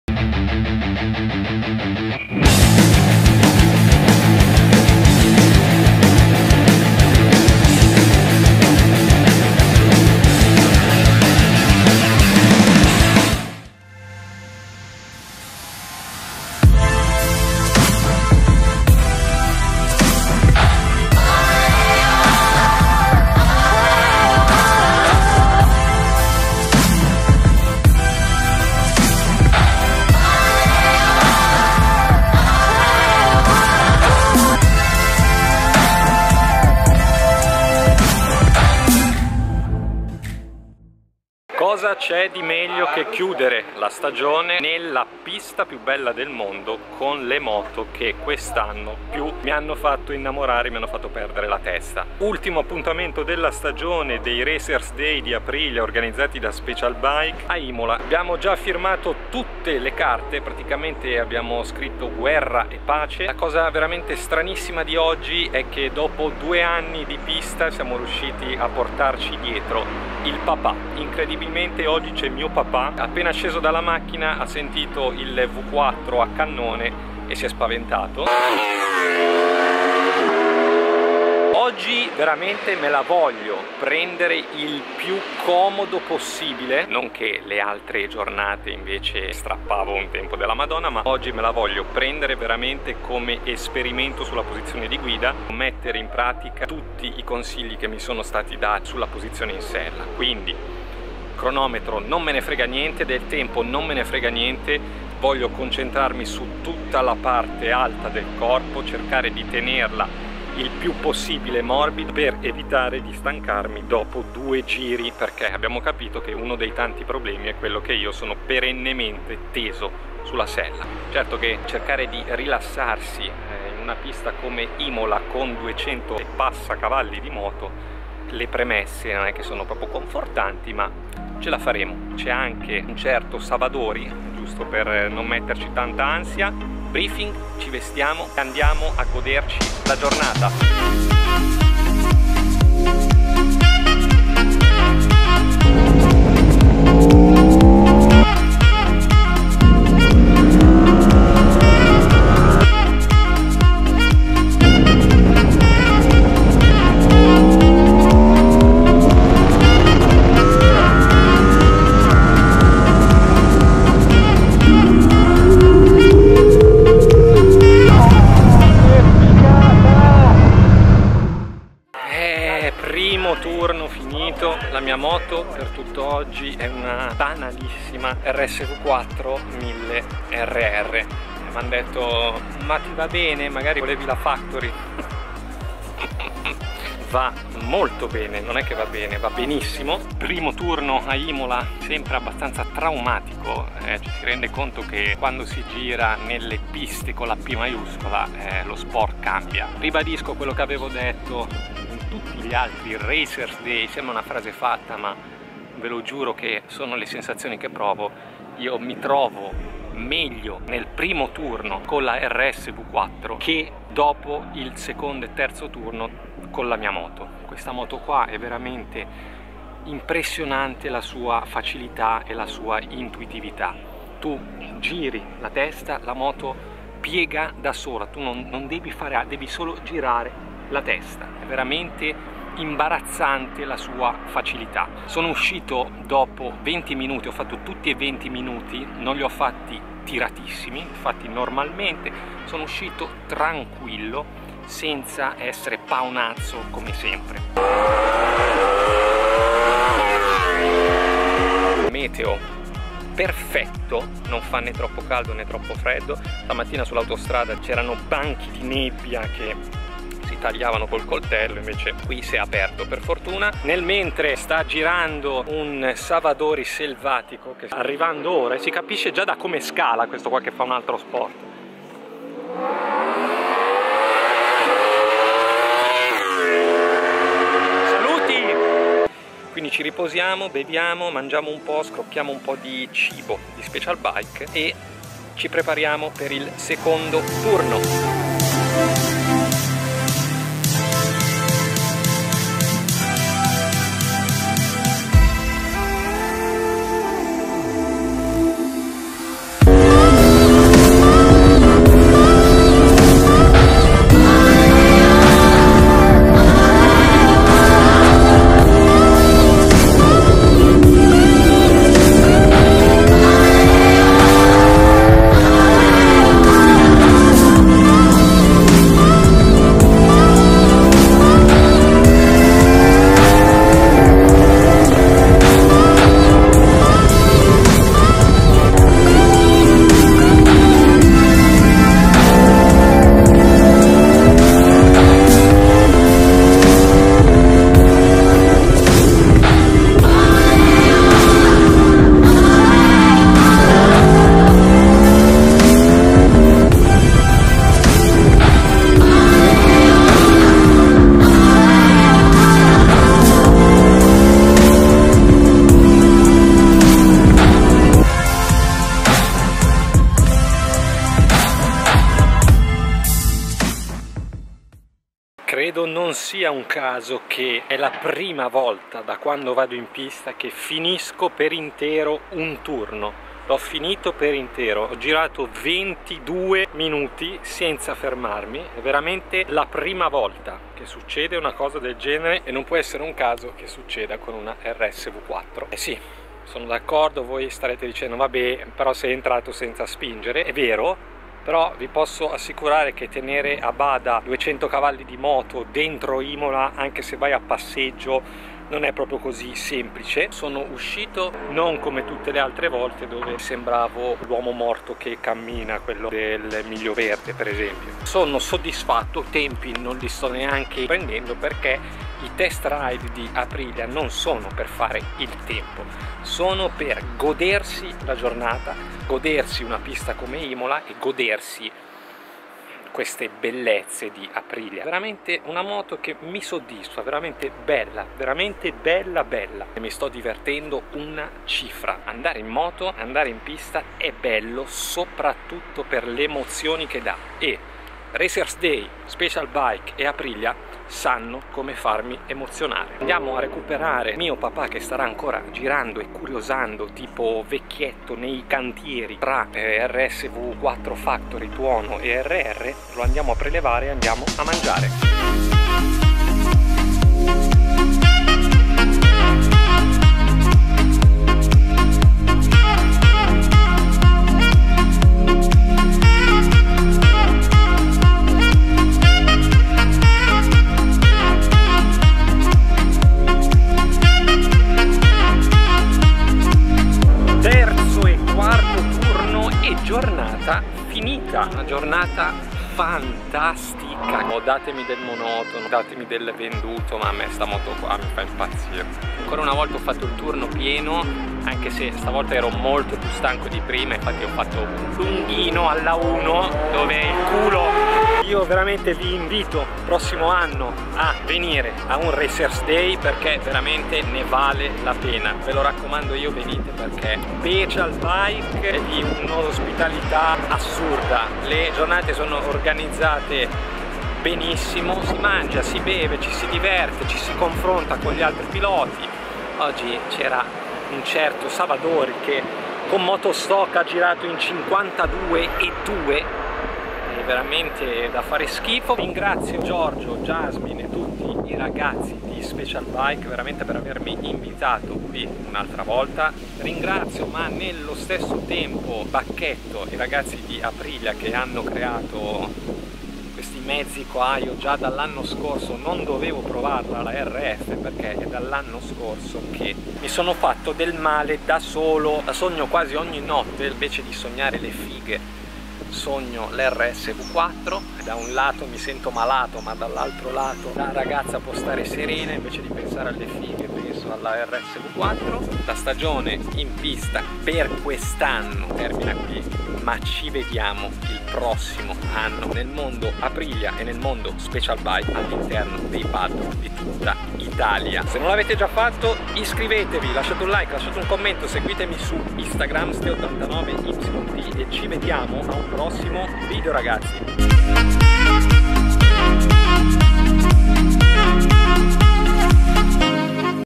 TU CREDI AL COLPO DI FULMINE? 💗 - APRILIA RACERS DAY EP. 4
Ultimo Racers Day della stagione in sella alla rsv4 di casa aprilia nella pista di imola.
Follemente innamorato di questa moto, più la guido e più mi convinco.